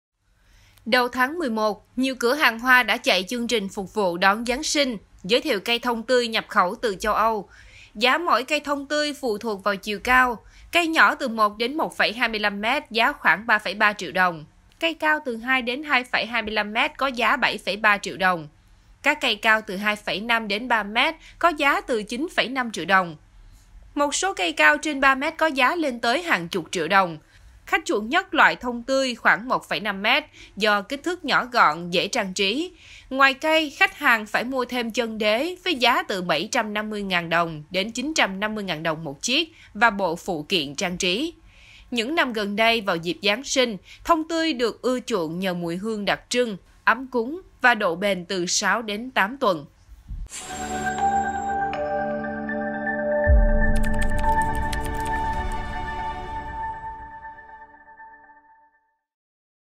Thông tươi chục triệu đồng về từ châu Âu phục vụ Giáng sinh
Đầu tháng 11, nhiều cửa hàng hoa đã chạy chương trình phục vụ đón Giáng sinh, giới thiệu cây thông tươi nhập khẩu từ châu Âu. Giá mỗi cây thông tươi phụ thuộc vào chiều cao. Cây nhỏ từ 1-1,25m giá khoảng 3,3 triệu đồng, cây cao 2-2,25m có giá 7,3 triệu đồng, các cây cao 2,5-3m có giá từ 9,5 triệu đồng.